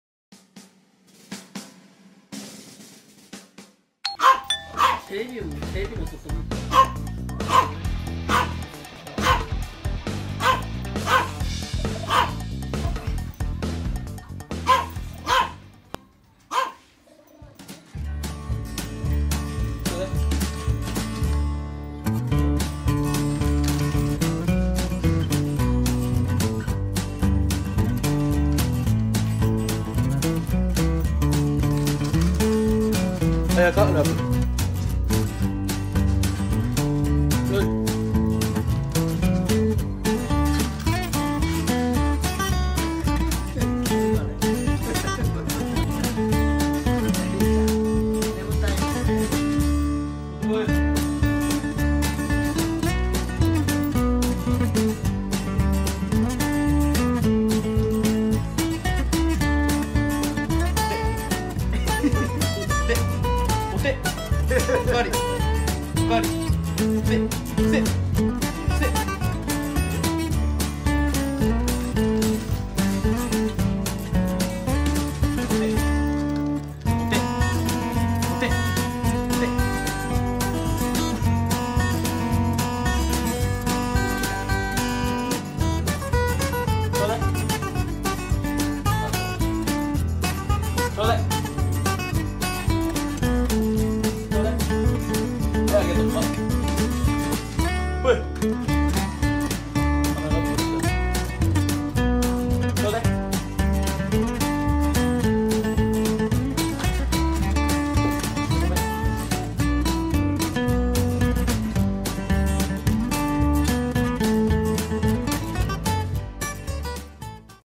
でっかいのが帰ってきた！どりらぶかむと戯れる【トイプードル】【多頭飼い】【トイプードルどりらぶかむ】
でっかい息子のおかえり！
両手で器用に別々の遊びをやってこなすwww
でっかくて気が優しいから小さいワンコにも優しい(◍︎ ´꒳` ◍︎)b
え？は？はい親バカ再び(〜￣▽￣)〜

愛犬 トリミング 、自分が犬と遊んでいる気分になれる動画 【トイプードル】【多頭飼い】【トイプードルどりらぶかむ】